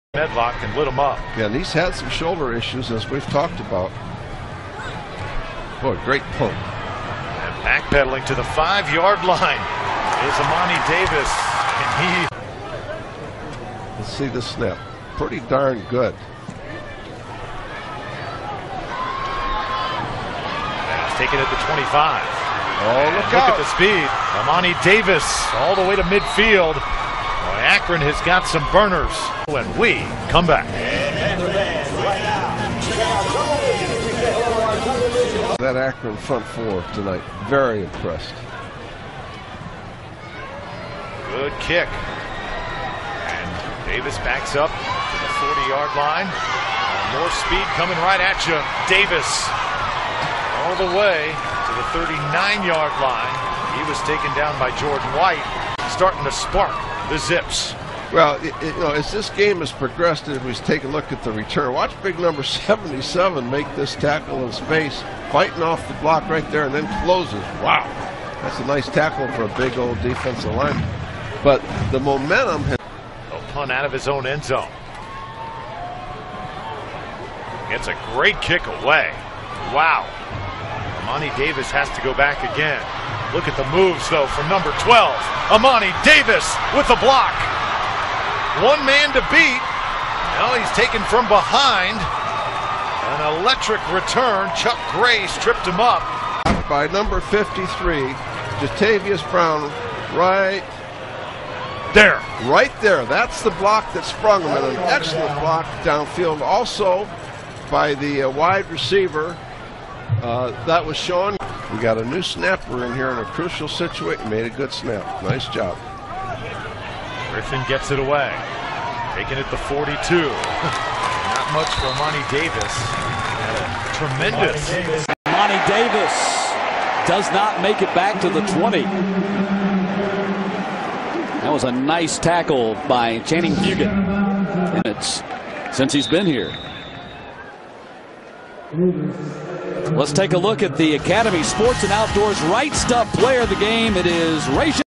Imani Davis - Akron Football - WR - Returning
Imani Davis FR WR Akron Kick and Punt Returning 2012 Highlights